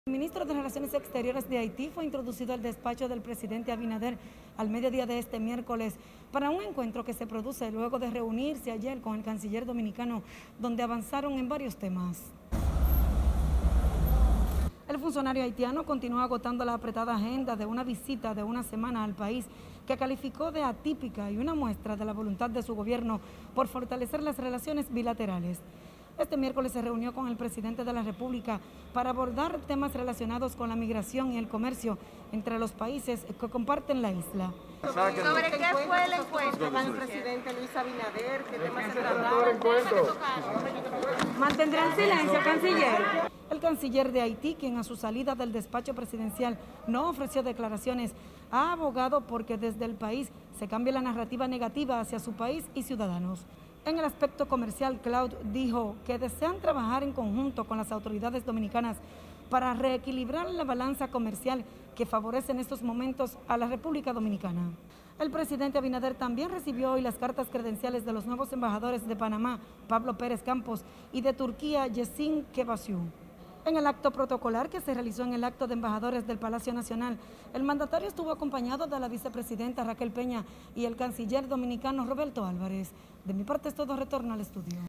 Canciller haitiano se reúne con Abinader
El presidente Luis Abinader se reunió hoy con el canciller de Haití, Claude Joseph, en el cuarto día de la visita oficial que realiza el importante funcionario para impulsar las relaciones bilaterales.